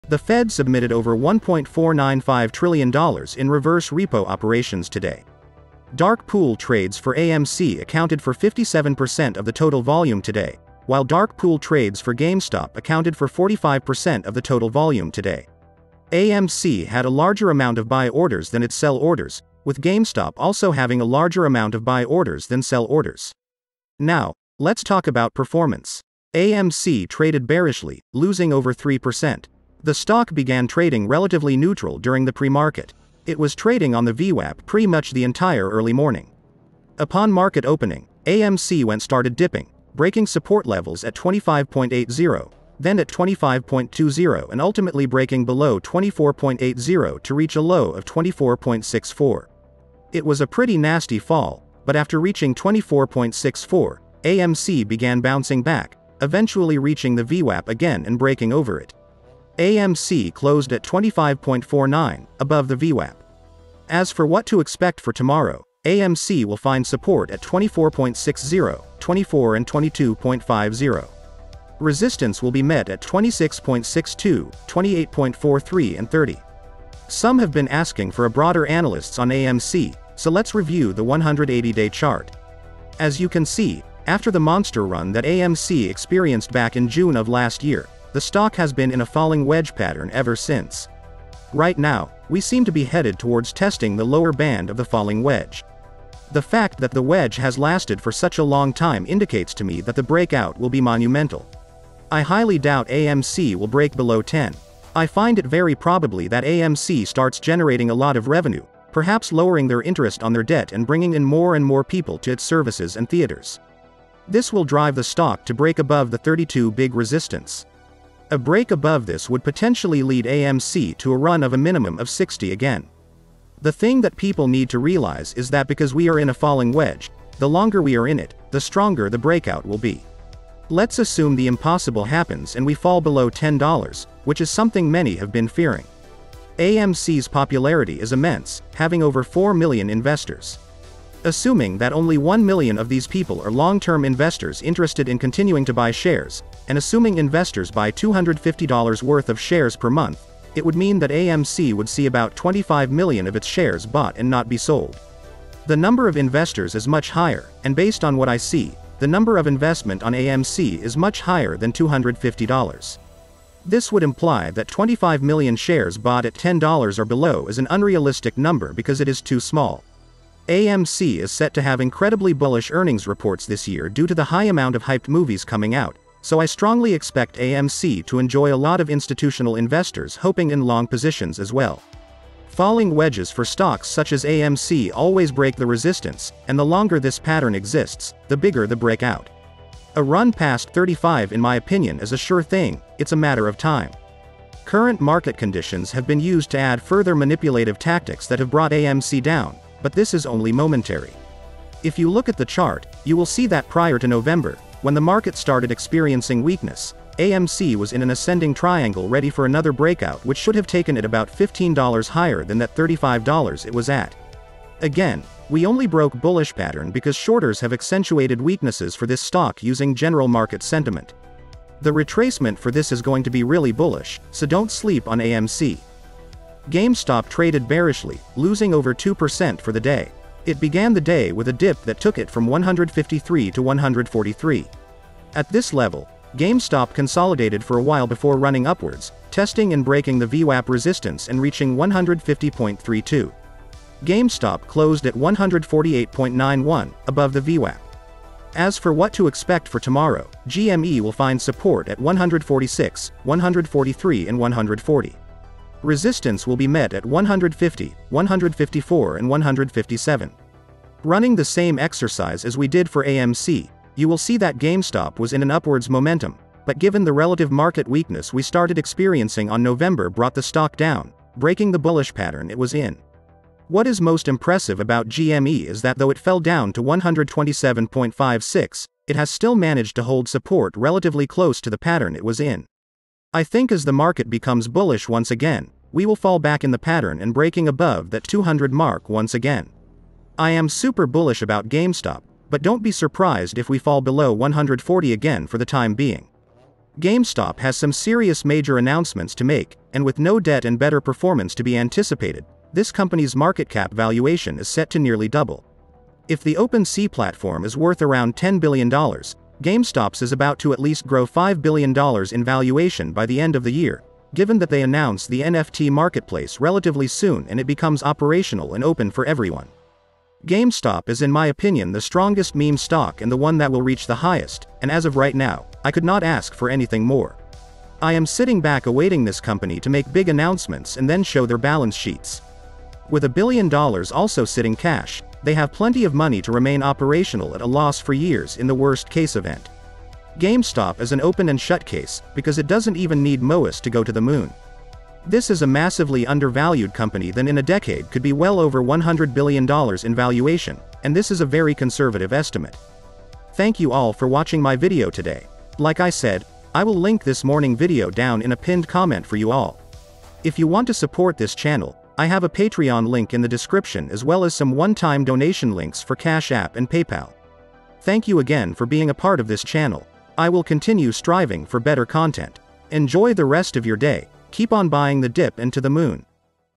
AMC Stock & GME Stock - What Happened Today? The Only Video You Need To Watch
AMC Stock and GME Stock are the best hedge against the current poor market conditions, and this video explains why GameStop and AMC are strong solid investment opportunities for everyone. It is intended to inform people about developments and theories all culminating into what I believe will be MOASS.